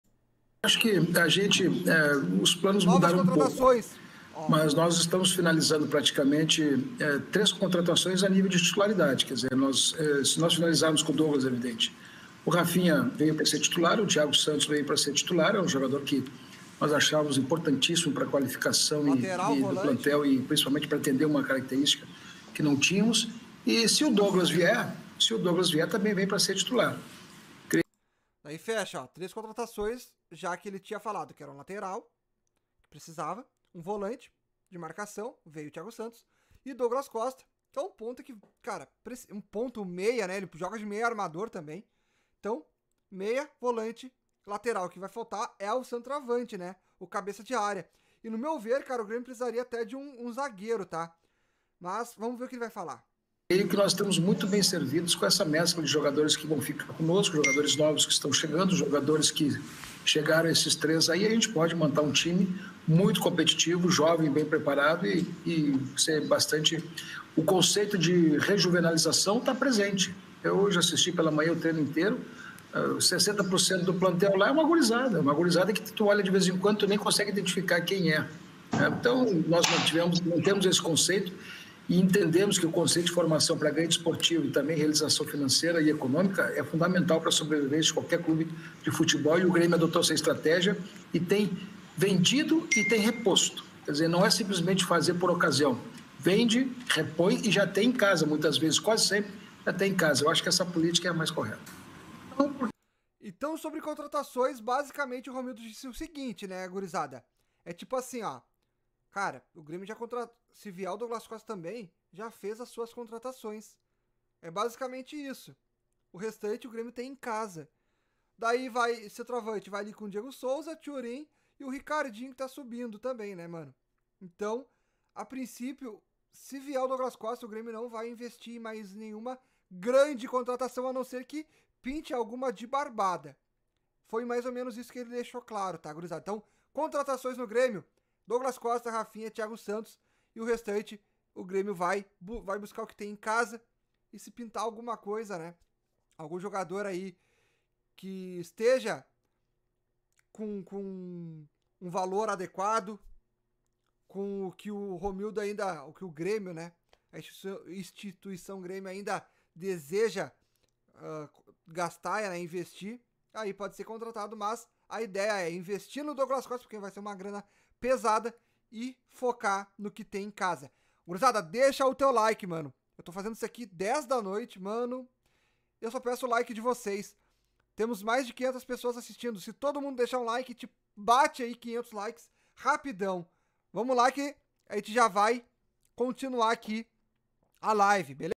MAIS REFORÇOS além de DOUGLAS COSTA? INFORMAÇÃO do PRESIDENTE ROMILDO!
Veja o que Romildo falou sobre mais reforços no Grêmio além de Douglas Costa!